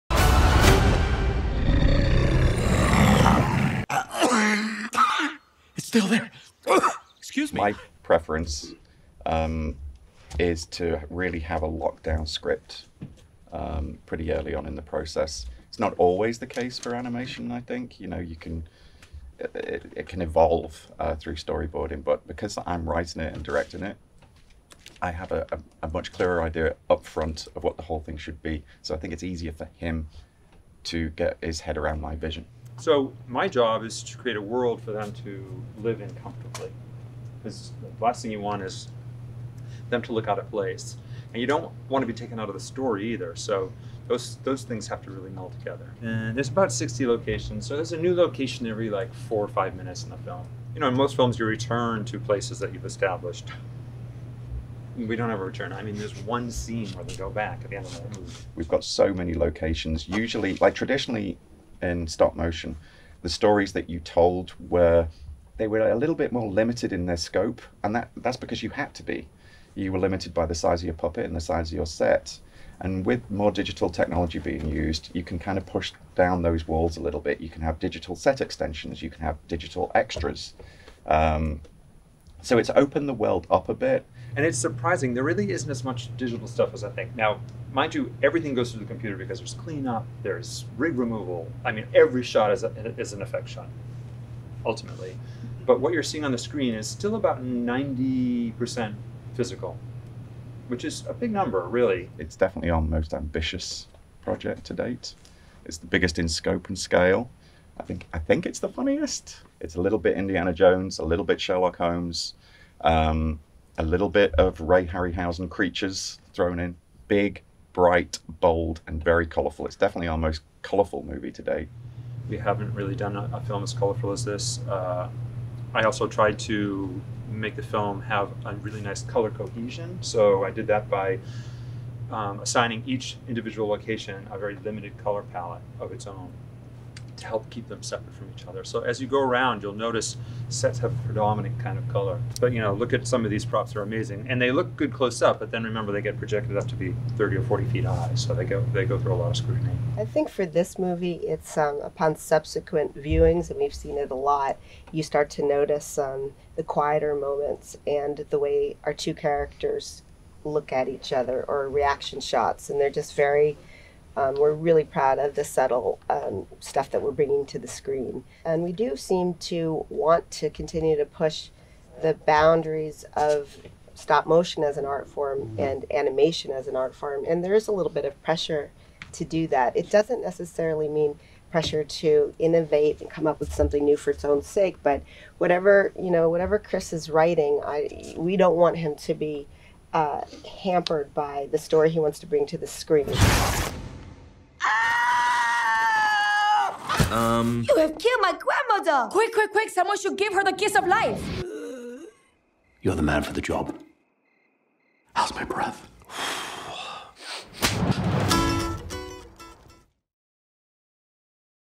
Laika takes us inside their studio for a look at The Missing Link
Missing Link, the new stop-motion animated feature from Laika, follows a lovable sasquatch (Zach Galifianakis) who, tired of his life in the Pacific Northwest, travels to the fabled valley of Shangri-La to find his long-lost relatives. Ahead of the film’s release this week, Laika invited us to their studio to show us how everything came together for the film